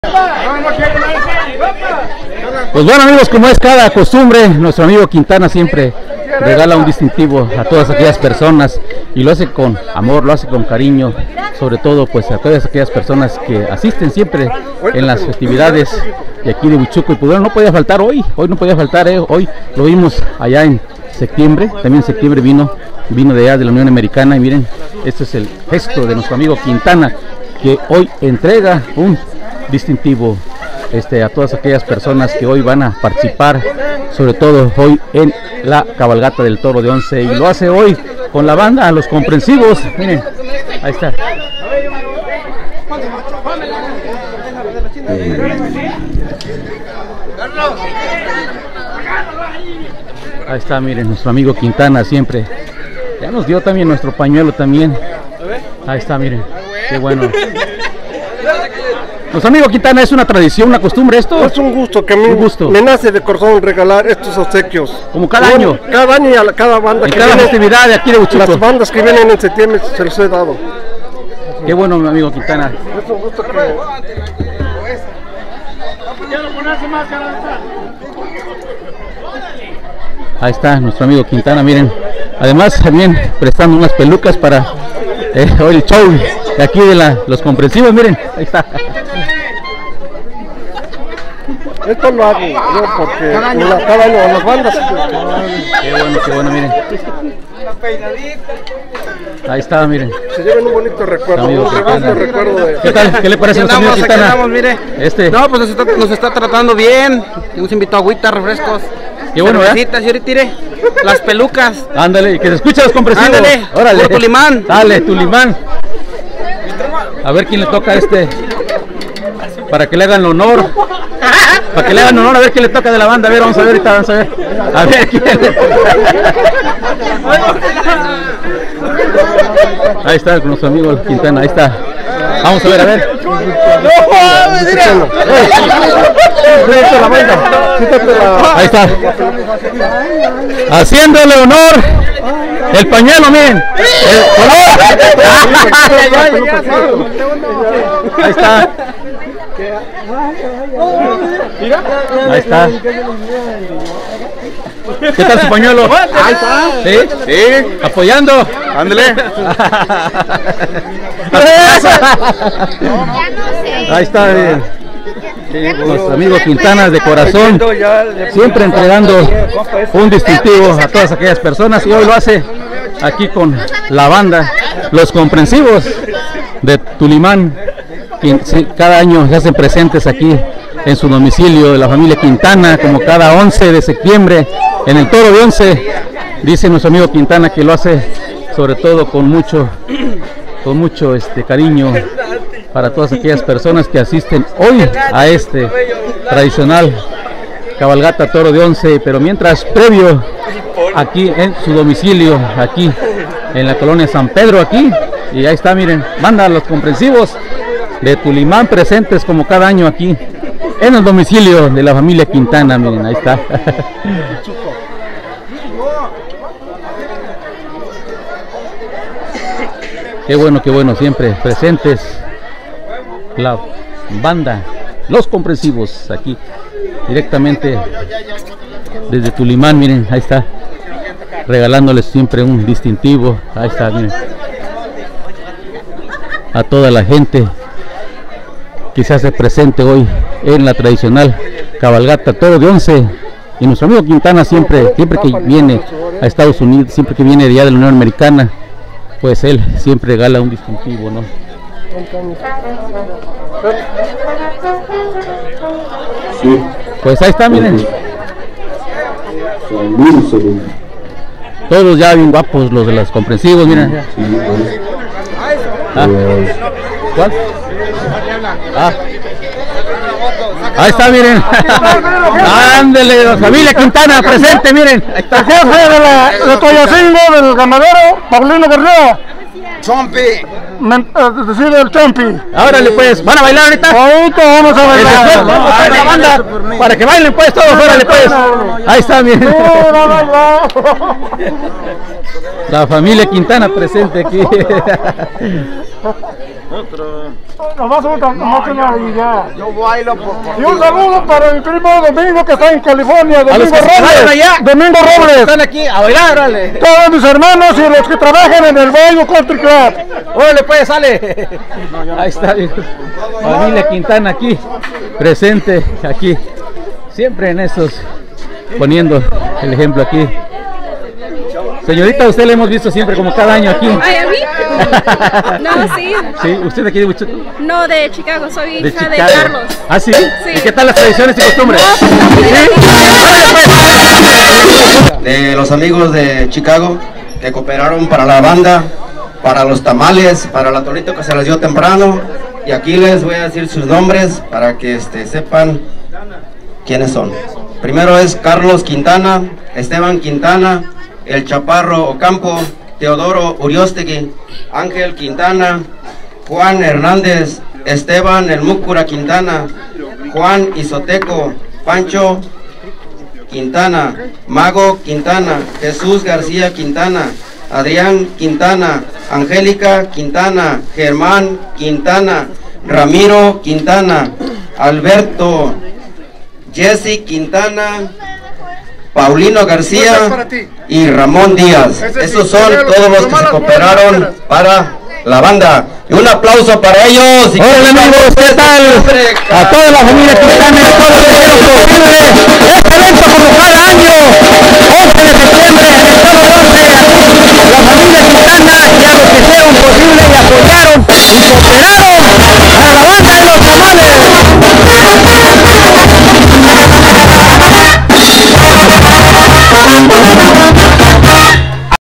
La Familia Quintana como cada año regala de corazón un presente 💝.
Como ya es constumbre la familia Quintana entrega un presente 💝 en cada festividad del pueblo.